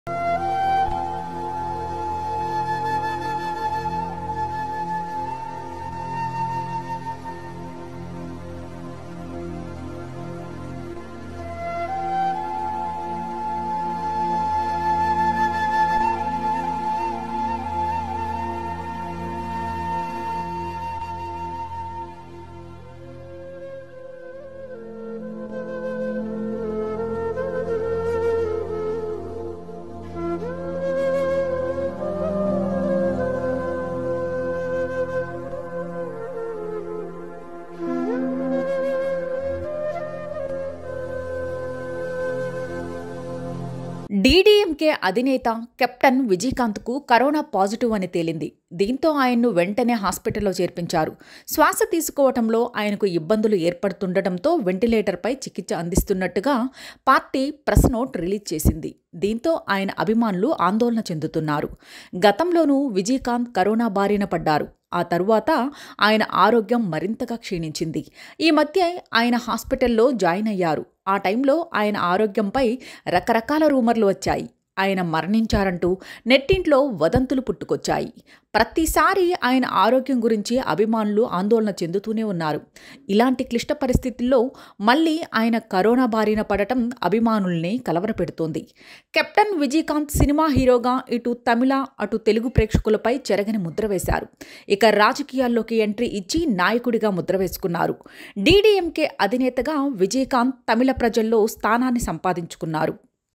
విజయకాంత్ కోసం భార్య & కొడుకు గుండెపగిలేలా ఏడ్చారు || Vijaykanth Son And Wife Crying || NS
Watch Here Vijaykanth Son And Wife Crying